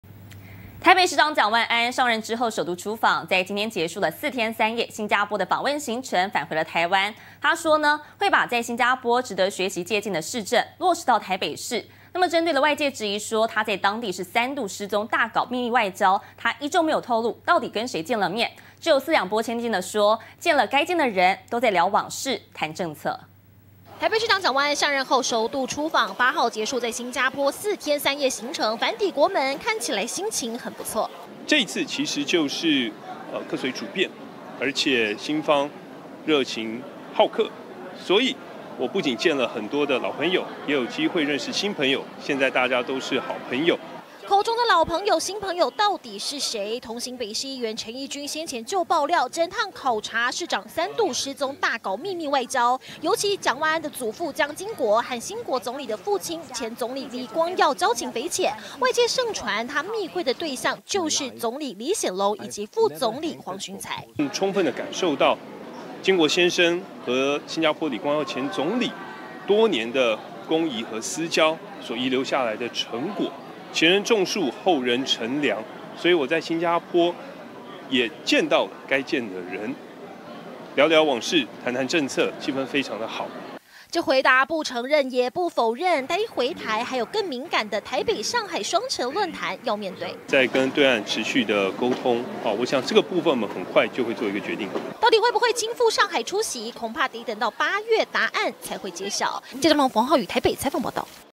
訪星遭爆3度失蹤？蔣萬安：見了很多老朋友
台北市長蔣萬安，上任後首度出訪，今天(8)結束 4天3夜新加坡訪問 返台，表示會把新加坡 值得 學習借鏡的市政，落實到台北市；針對外界質疑，他在當地「三度失蹤」大搞「秘密外交」，他依舊 沒透露 到底密會了誰，四兩撥千金 感謝祖父蔣經國，和星國總理李光耀 多年公誼私交 留下來的成果，所以「見了該見的人、都在聊往事、談政策」。